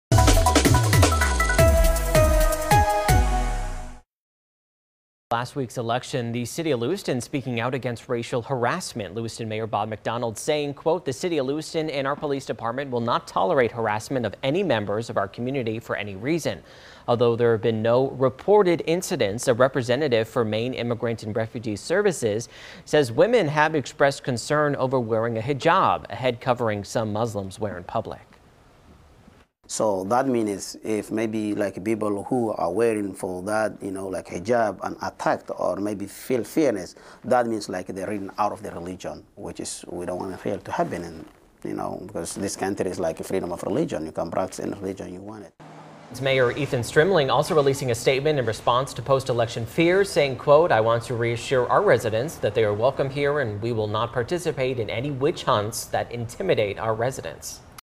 Lewiston, Portland leaders speak out about racial harassment
Leaders in Lewiston and Portland are speaking out about harassment targeting Maine's Muslim community.